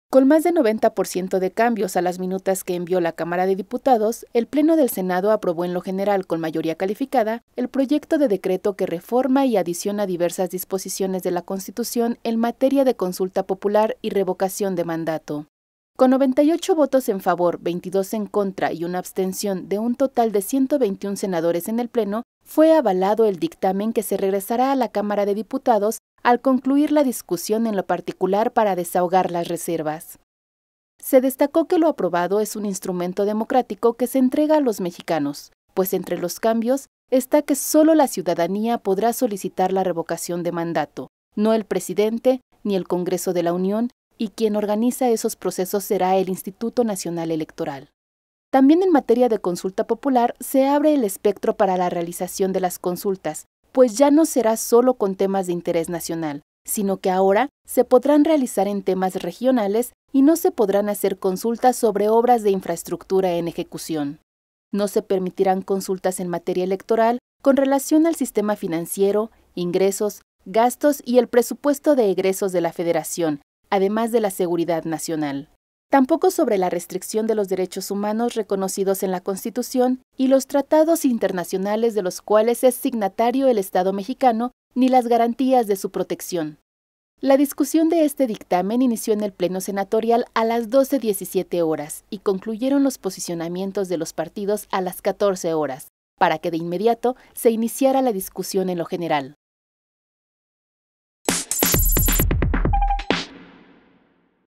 Senado da luz verde a revocación de mandato, con ajustes
Con 98 votos en favor, 22 en contra y una abstención, de un total de 121 senadores en el Pleno, fue avalado el dictamen, que se regresará a la Cámara de Diputados al concluir la discusión en lo particular para desahogar las reservas.